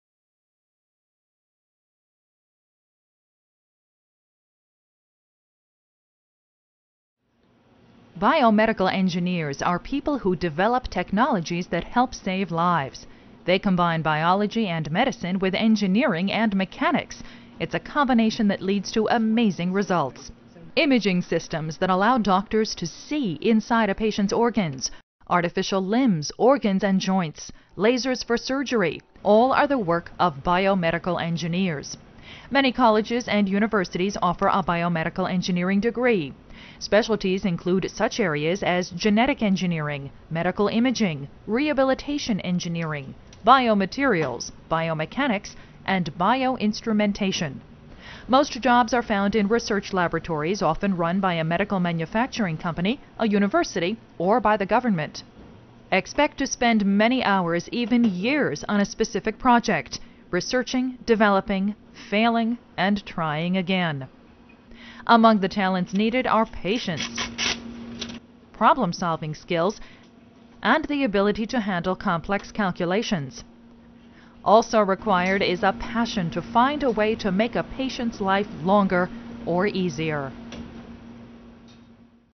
What Does A Biomedical Engineer Do?
Learn more about what a Biomedical Engineer does. Explore a career as a Biomedical Engineer including work environment, salary, career outlook and degree options